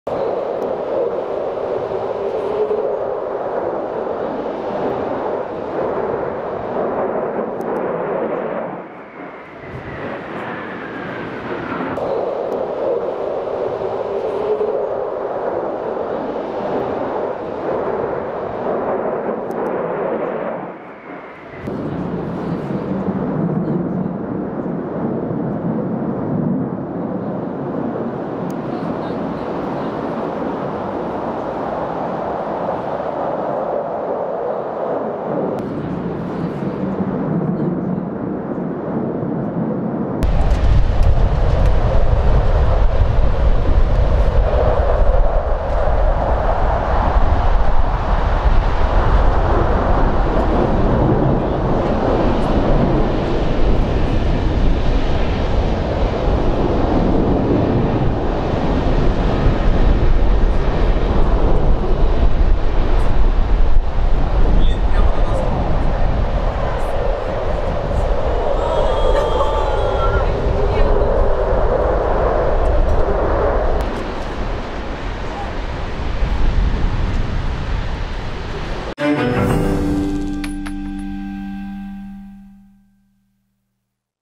«Русские витязи» - репетиция в Ижевске
Рев турбин тяжелых истребителей напугал в четверг, 19 октября, жителей Ижевска. Оказалось, что в столицу Удмуртии прилетели два самолета пилотажной группы «Русские Витязи»